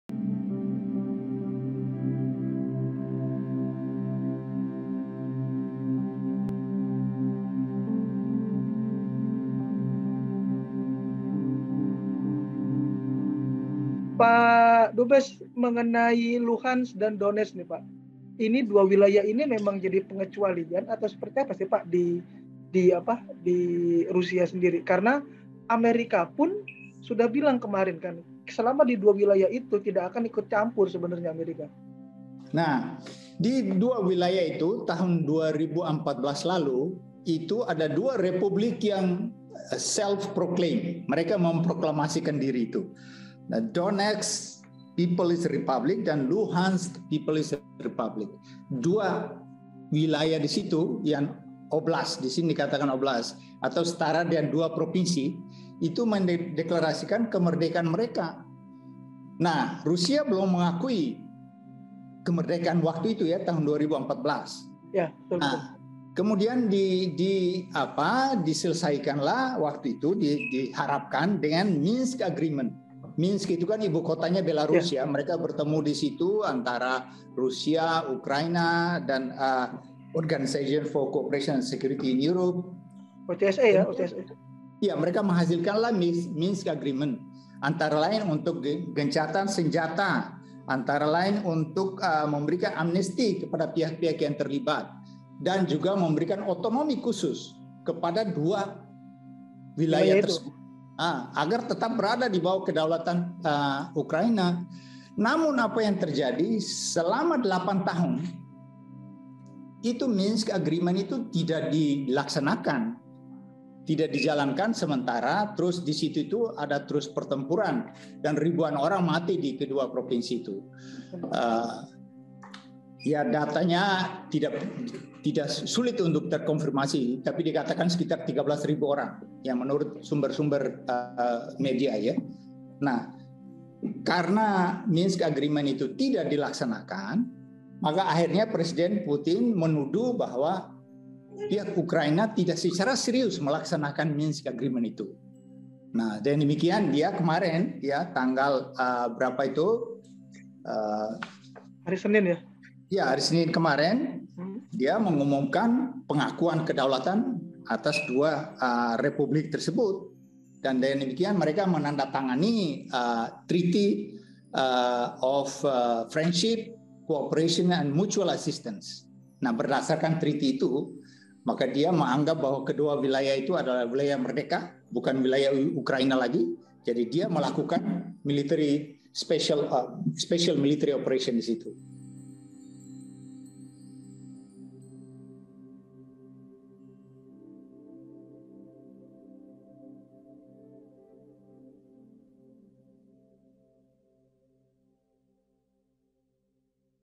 Rusia vs Ukraina, Ini Penjelasan Dubes Jose Tavares Part 3: Situasi Sebelum Serangan
Ada sejumlah hal yang akhirnya menjadi pemicu konflik Rusia dan Ukraina di perbatasan. Salah satunya diceritakan Dubes RI untuk Rusia, Jose Tavares dalam wawancara dengan VOI, Kamis, 24 Februari.